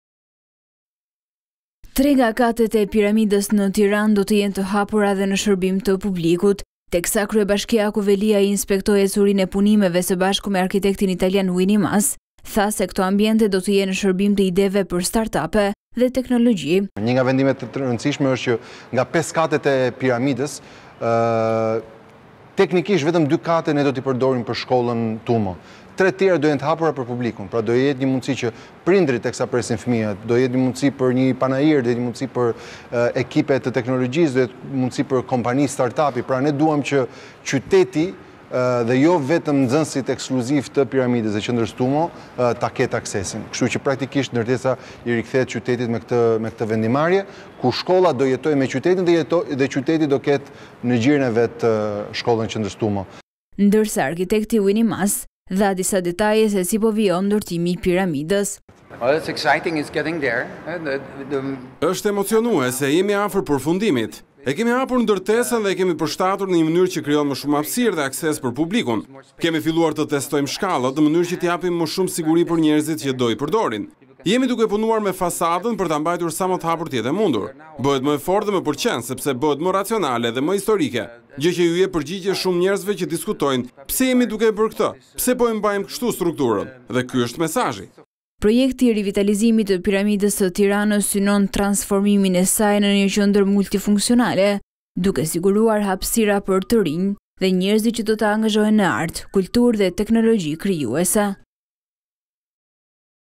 Veliaj: Tre katet e Piramidës do jenë të hapura për publikun - Vizion Plus
3 nga katet e Piramidës në Tiranë do të jenë të hapura dhe në shërbim të publikut.Teksa kryebashkiaku Veliaj inspektoi ecurinë e punimeve së bashku me arkitektin Winy Mass, tha se këto ambiente do të jenë në shërbim të ideve për start-up dhe teknologji. “Një nga vendimet e rëndësishme është se nga 5 katet e Piramidës, vetëm dy kate do t’i përdorim për shkollën Tumo, tre të tjerat do të jenë të hapura për publikun. Pra, do të jetë një mundësi që prindërit teksa presin fëmijët, te shohin një panair, ose te perdoret nga ekipet e teknologjisë ose nga kompani start-up. Ne duam që i gjithë qyteti të ketë akses te Piramida. Kështu që, praktikisht ndërtesa i rikthehet qytetit. Për sa i përket fasadës, të gjithë fëmijët e viteve 80 dhe 90-të, duan atë rrëshqitesen e famshme, por disa janë mësuar me festivalin e filmit dhe me kinemanë verore, por edhe gjelbërim”, Ndërsa Arkitekti Winy Mass dha disa detaje më specifike se si po vijon ndërtimi i Piramidës. Është emocionuese, jemi afër përfundimit. E kemi hapur ndërtesën dhe e kemi përshtatur në një mënyrë që krijon më shumë hapësirë dhe akses për publikun. Kemi filluar të testojmë shkallët, në mënyrë që të japin më shumë siguri për njerëzit që do i përdorin. Jemi duke punuar me fasadën për ta mbajtur sa më të hapur të jetë e mundur. Bëhet më e fortë dhe më pëlqen sepse bëhet më racionale dhe më historike gjë që iu jep përgjigje shumë njerëzve që diskutojnë pse jemi duke e bërë këtë, pse po e mbajmë kështu strukturën dhe ky është mesazhi. Projekti i rivitalizimit të Piramidës së Tiranës synon transformimin e saj në një qendër multifunksionale në qytet, duke siguruar hapësirë për të rinjtë dhe njerëzit që të angazhohen në art, kulturë dhe teknologji krijuese.